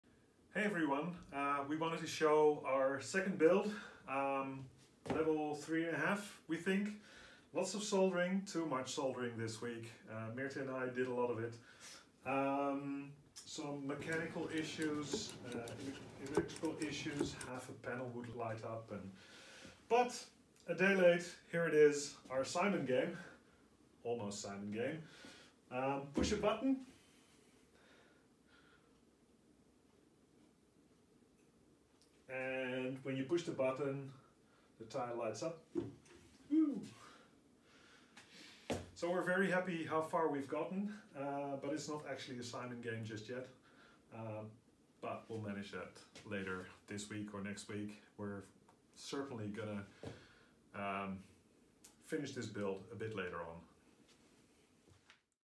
An Almost Simon Game
A short clip about our second build for the Creative Engineering with Mark Rober course on Monthly.com.

This is our somewhat presentable Simon game using lots of WS2812 LEDs using a Teensy 3.5 (just shy of 1300), lots of soldering and a bit of code. We're very happy with the results so far.